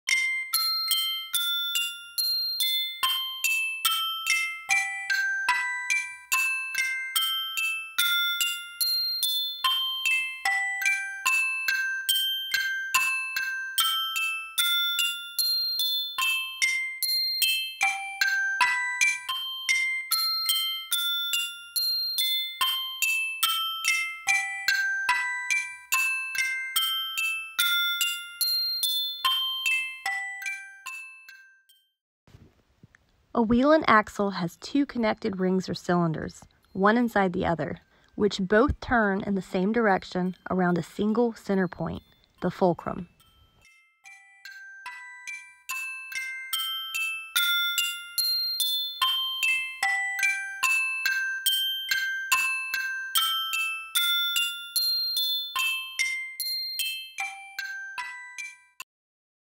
Quiver - Ferris Wheel
It will help students learn about simple machines & how they are used and applied on a daily basis. The students can enjoy the AR experience, and exciting games and learn about different parts of the Ferris Wheel – including the wheel, axel, carriage, and fulcrum!